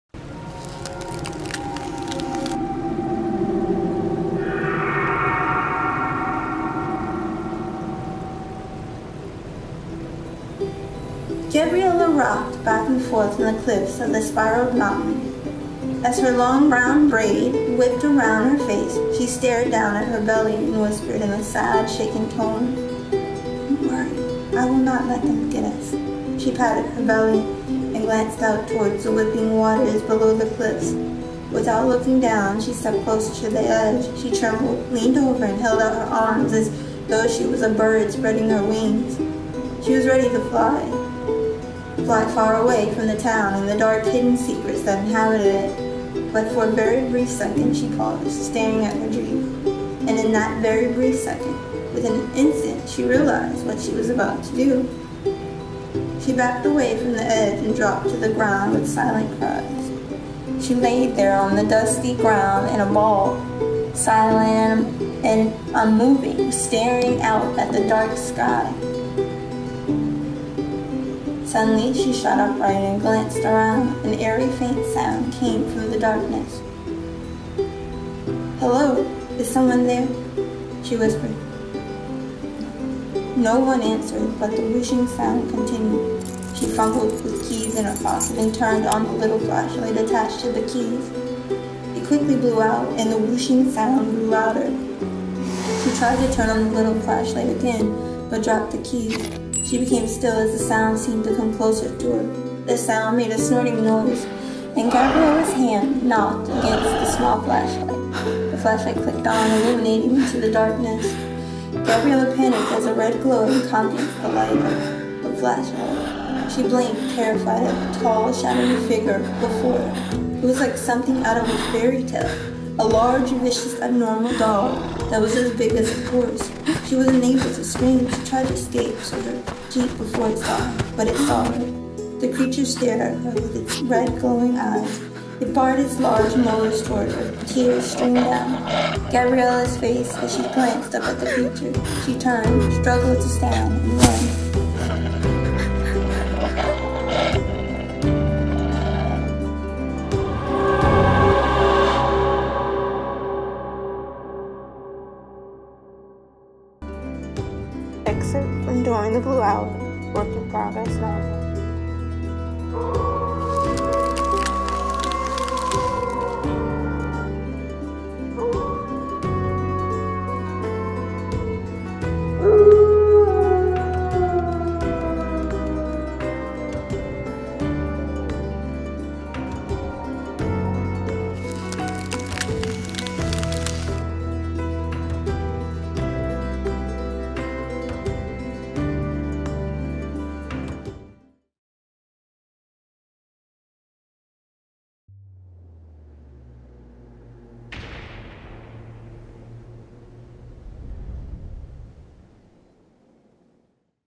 (Audiobook Preview Book 1 Werewolf Novel )- During the Blue Hour -Prologue: Magical World of Wonder
Werewolf Novel Audiobook Preview Book 1
Gabriela gets chased by an animal in the mountains. To her it seems to be a creature from fairytales and legend; just a big giant wolf? or werewolf?

This is the prologue chapter snippet from  my work in progress werewolf novel During the Blue Hour.

I'm almost finished  completing my 2nd draft  so each Monday around 5:00-5:30 I will  release a chapter  snippet  of what I have written and completed. Please leave your questions , and feedback in the comments below .